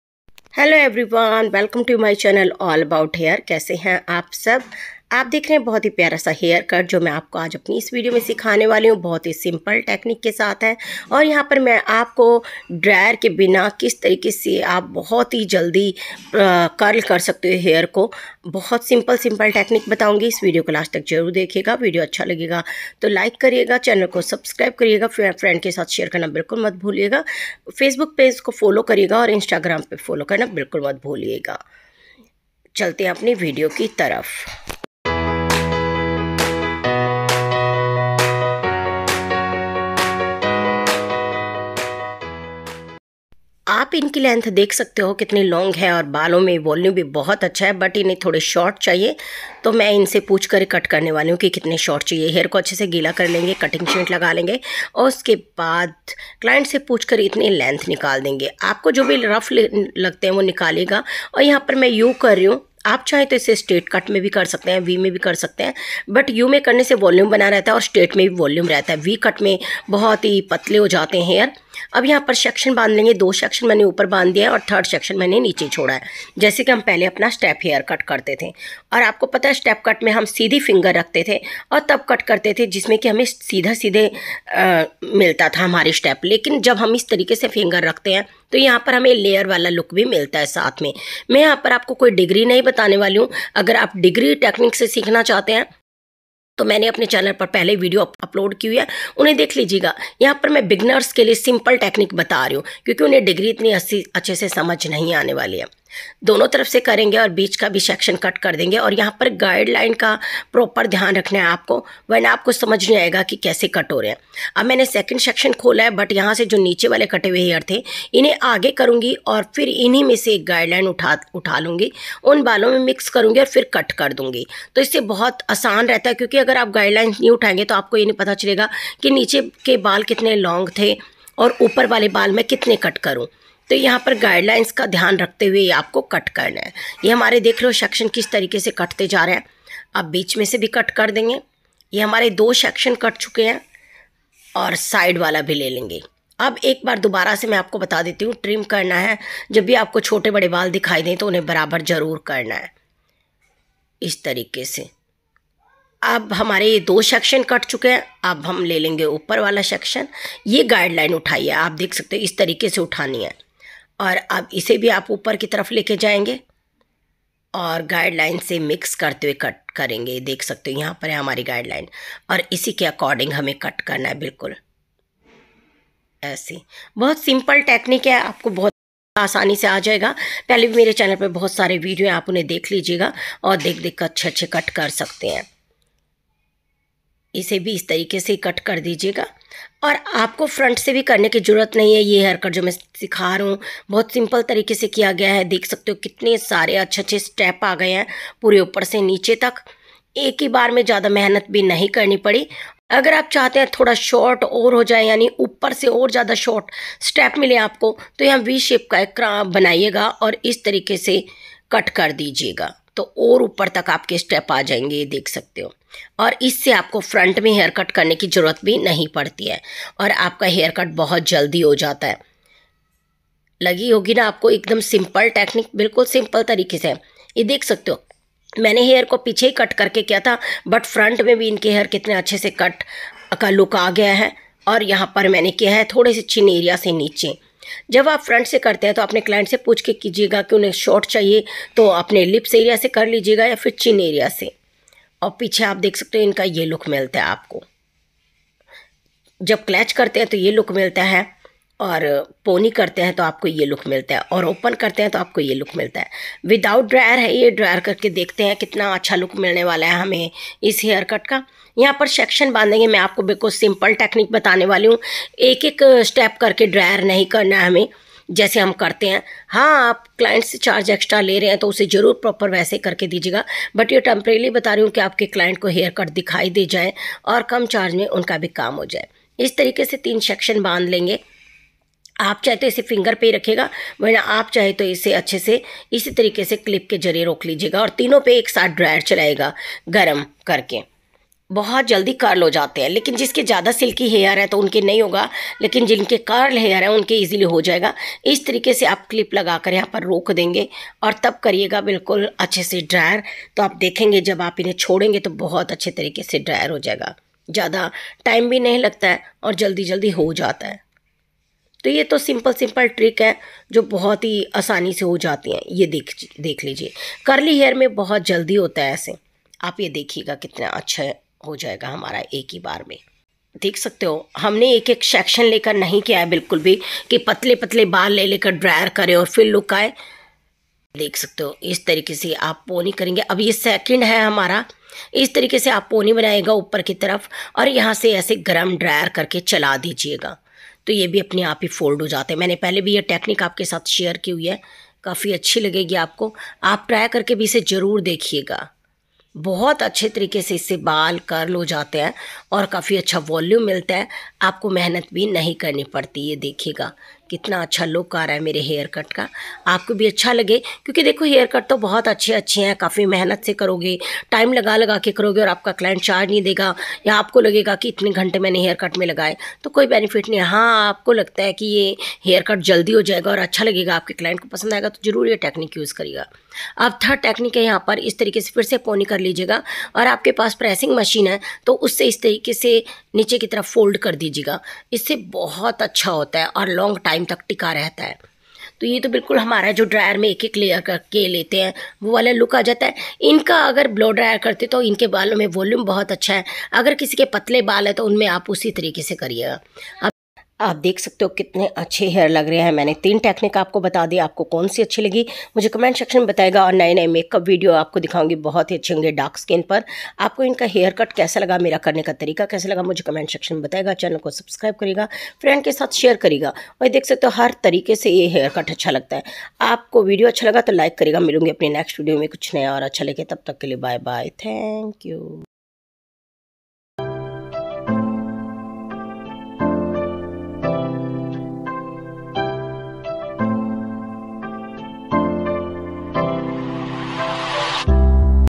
Advance multi Step Hair Cut For Beginners | Simple Hair Cut For Girls | @allabouthairs3111​
Aaj ke video main, main aapse share karne wali hu How to Simple Cut Your Hair ang to Curl Your Hair Without Using Hair Dryer . Toh pura video dekhiye aur bataye aapko kaisa laga